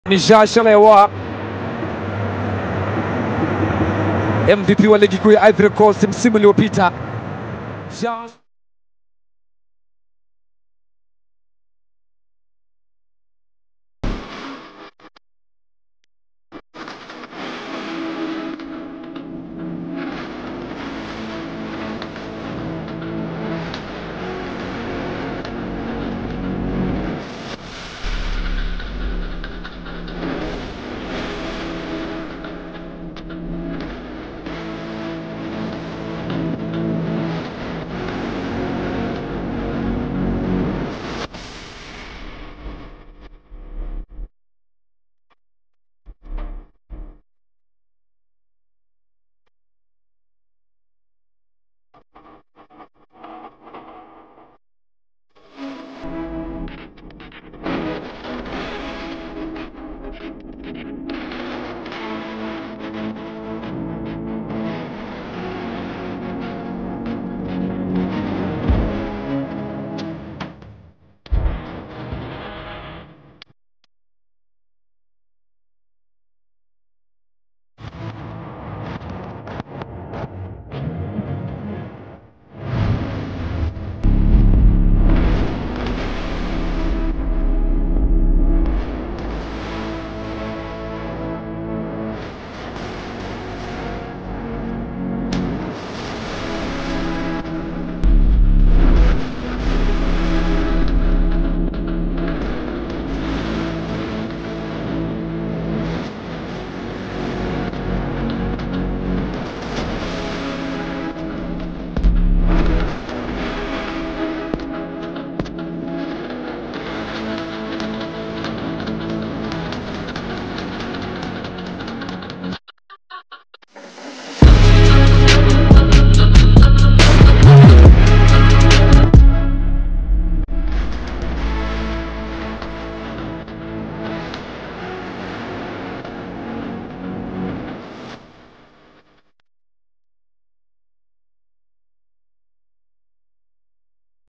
CHARLES AHOU UWEZO WAKE JUU YA QPR MATUKIO YOTE
matukio ya mchezaji wa simba Charles ahou ambaye ame onesha ubora wa hali ya juu simba vs qpr Charles ahou ni miongoni mwa walio ipa simba ushindi simba day